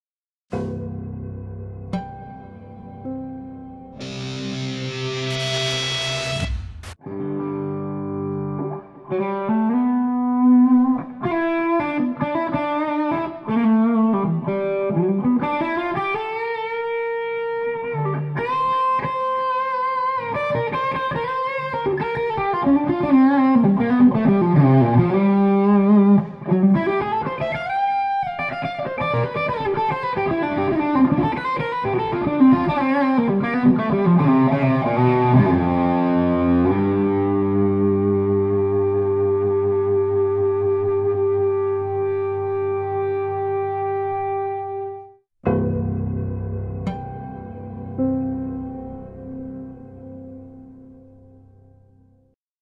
Suhr Custom Classic T Daphne Blue HH Electric #21807 Quick n' Dirty
In this video, Sound Pure electric guitar specialist Eddie Berman tests out a used Suhr Custom Classic T Daphne Blue HH Electric with an Ace Pepper Super Sport 40W Head, an Ace Pepper 1x12 w/ ASW Speaker Cabinet, and a Strymon El Capistan dTape Delay Effect Pedal.

This gear is available for our exclusive, free demo Try Before You Buy program.